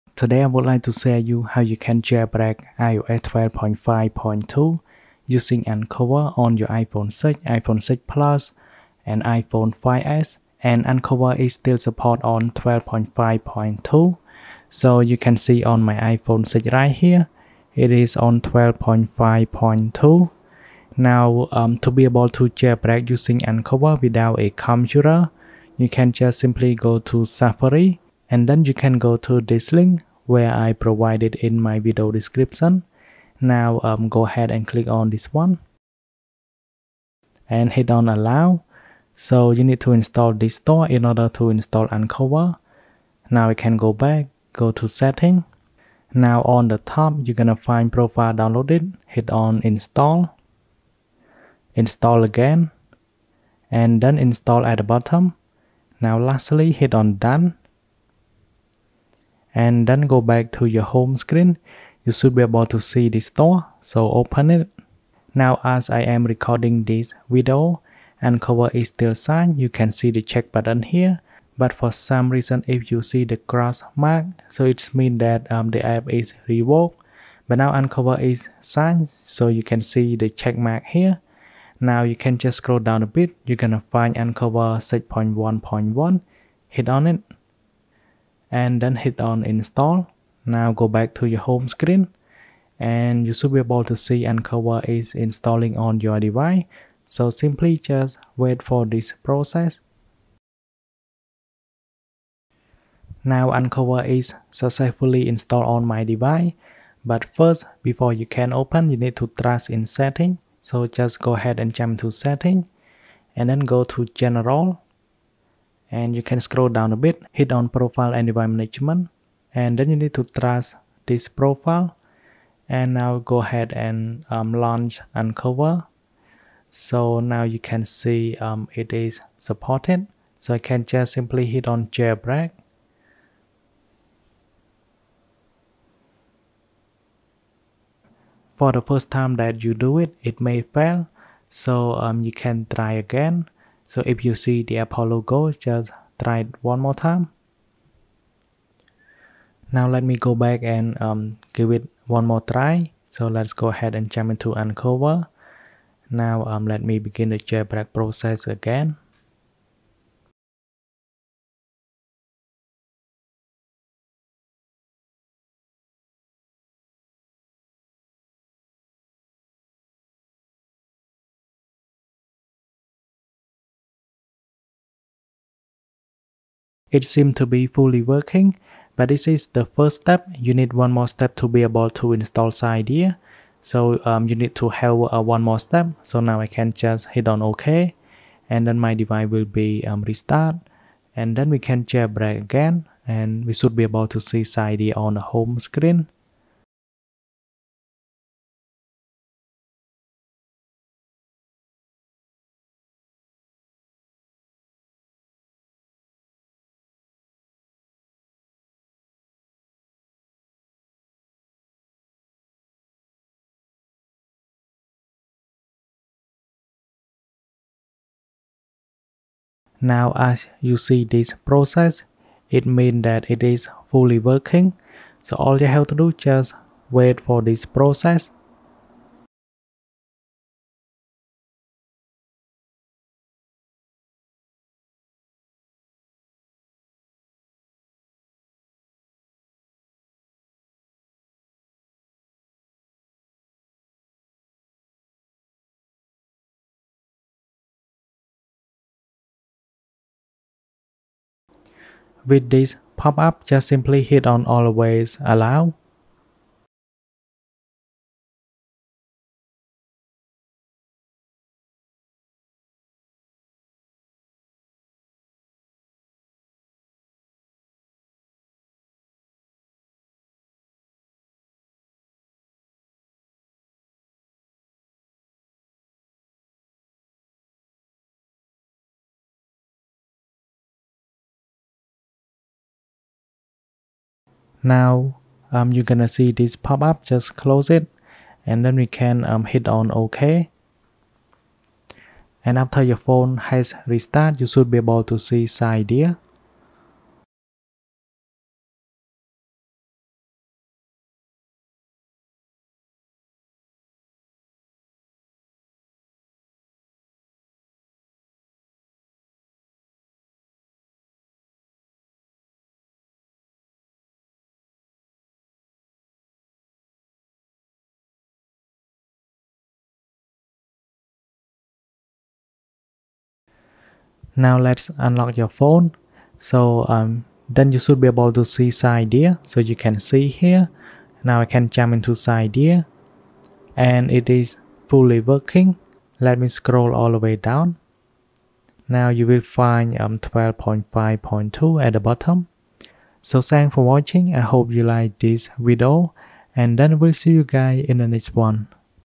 How to Jailbreak iOS 12.5.2 on iPhone 6/6plus/5s/iPadMini2 - No Computer
iOS 12.5.2 has just released. In this video, I would like to show you how to jailbreak iOS 12.5.2 on iPhone 6, iPhone 6 Plus, iPhone 5s, and iPad mini 2 No Computer Required